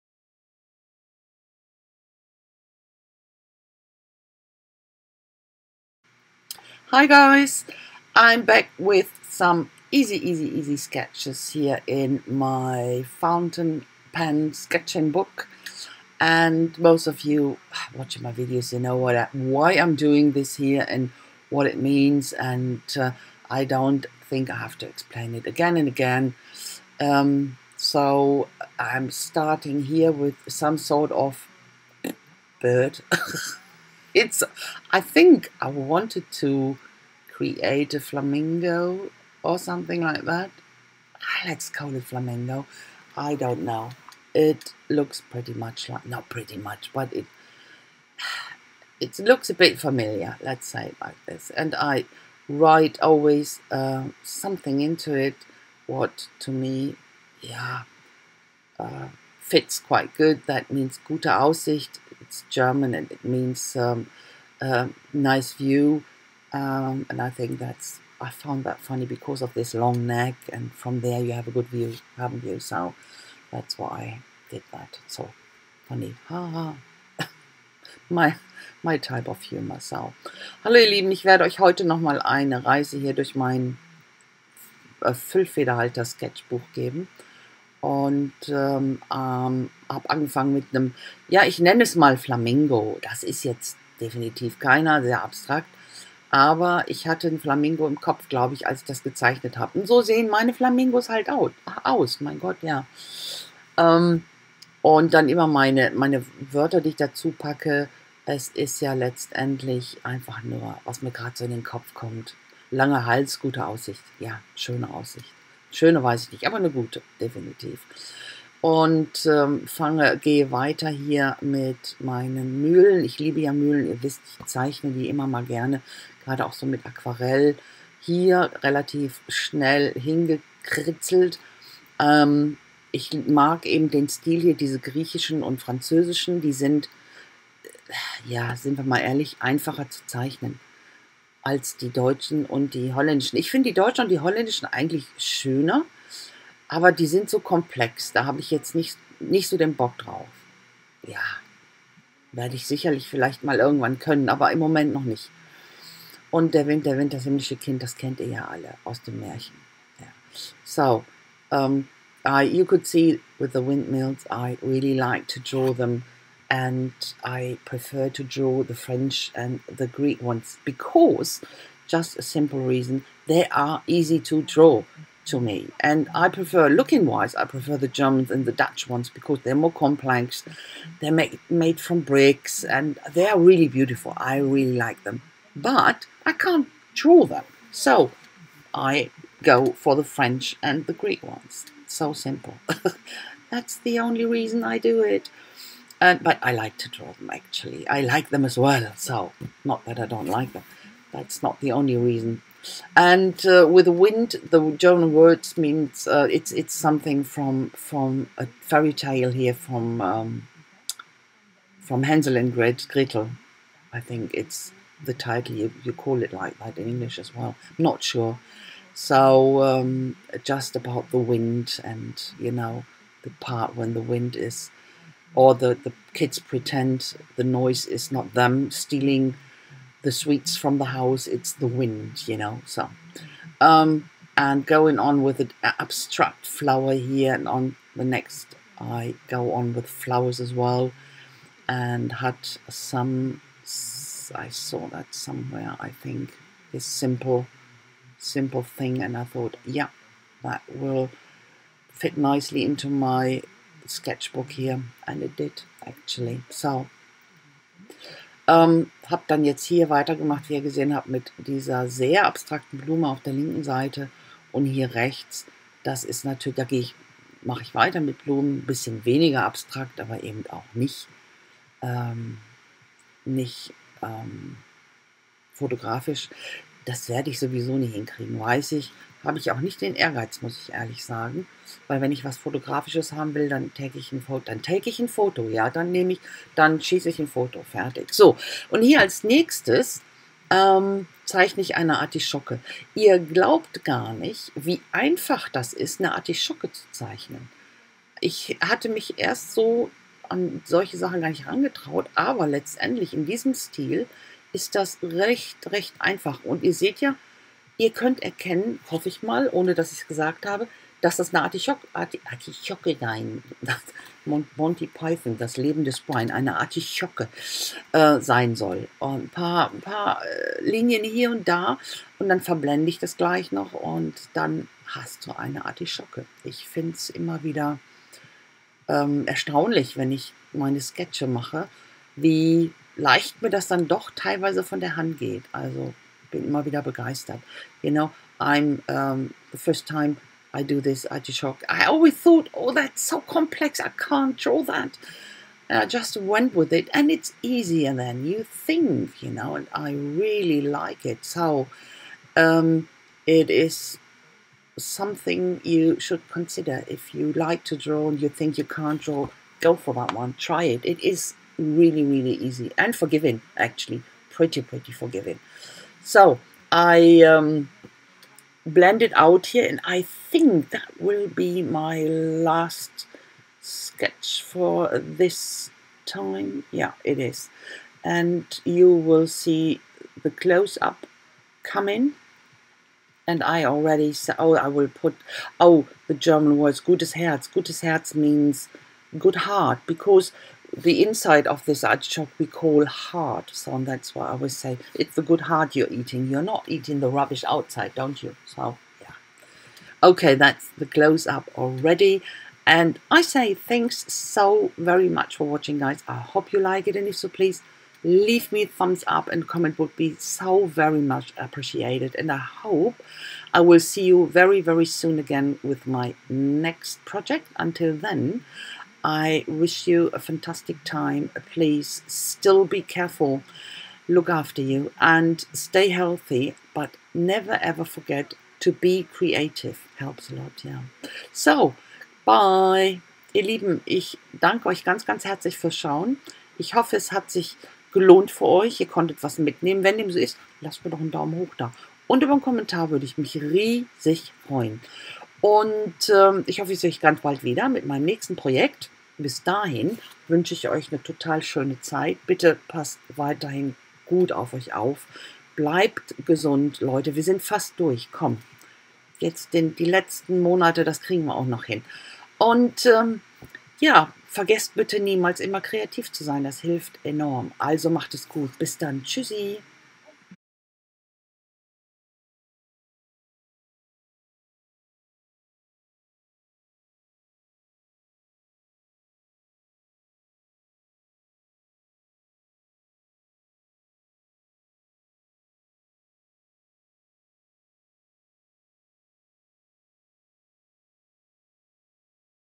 Easy sketching with a fountain pen
Practise to sketch with a fountain pen so that I can´t erase.